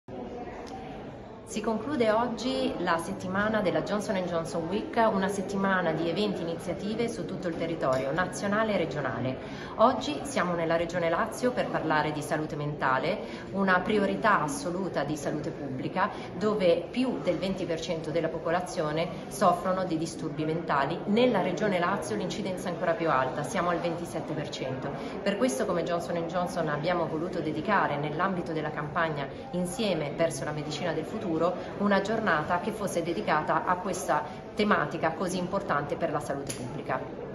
ENTERPRISE Monica Gibellini, Johnson&Johnson
Si è conclusa la “J&J week, insieme verso la medicina del futuro” con un incontro dedicato alla salute mentale nella regione Lazio.
Un commento di Monica Gibellini, Direttrice Government Affairs, Policy & Patient Engagement, Johnson & Johnson Innovative Medicine